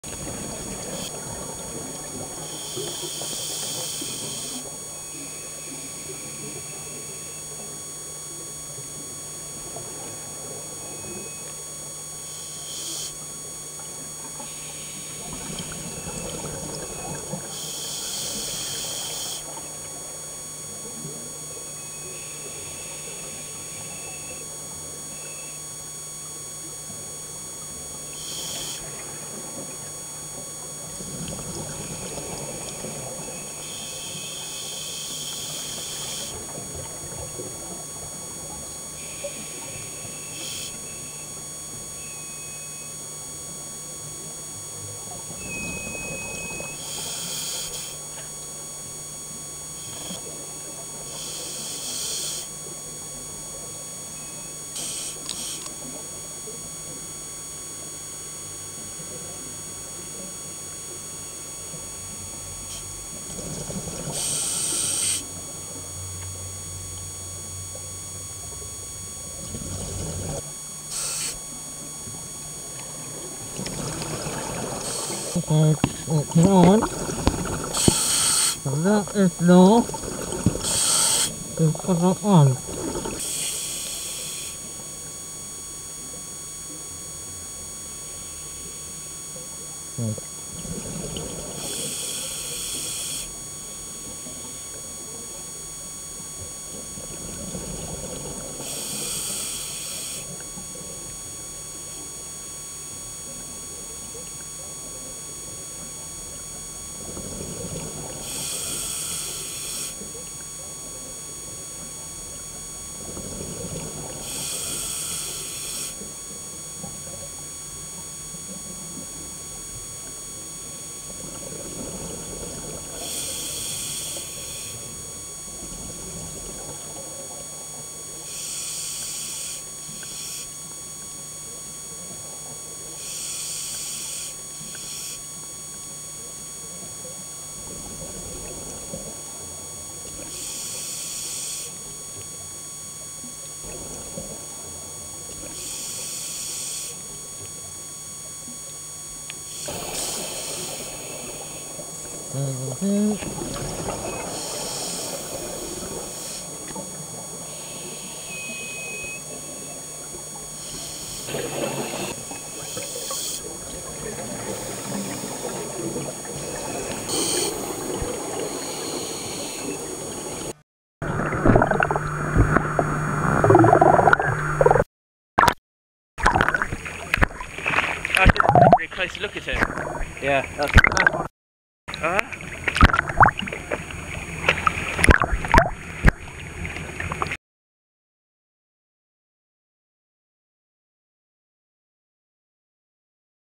Rare sea slug found in North Wales
The Marine Monitoring Team at the Countryside Council for Wales (CCW) use video to record samples of biogenic horse mussel reef off the North Llŷn Coast.  Due to the depth of the site (approx. 35m) divers have limited time and therefore quadrats are recorded on video and analysed later.  This footage shows a biogenic reef teeming with life including horse mussels Modiolus modiolus, brittlestars, soft coral Alcyonium digitatum and a curled octopus Eledone cirrhosa.  A rare species of sea slug, the yellow skirt slug Okenia elegans was also captured on film.  This is only the second recorded sighting in waters around North Wales.  Further information about this species can be found